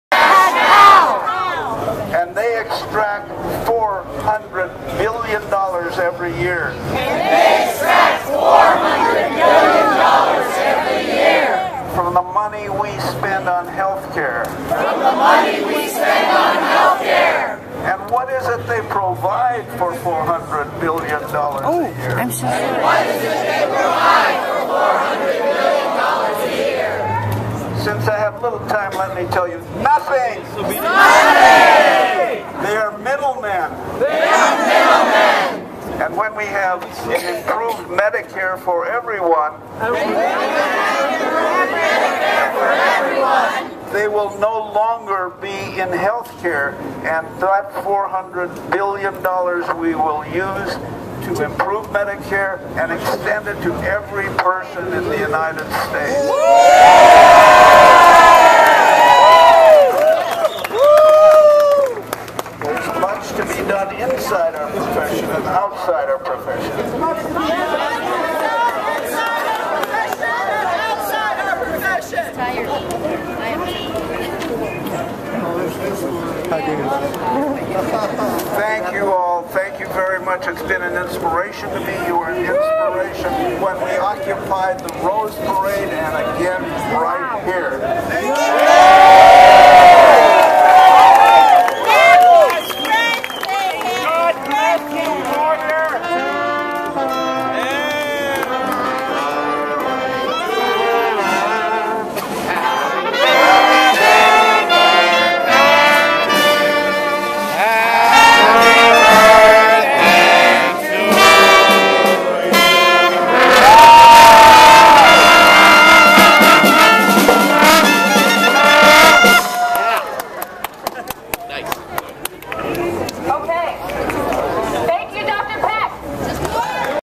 Dr. Robert Peck speaks at Occupy J9 rally
J9 Occupy Anthem Blue Cross - Happy Birthday Dr. Peck!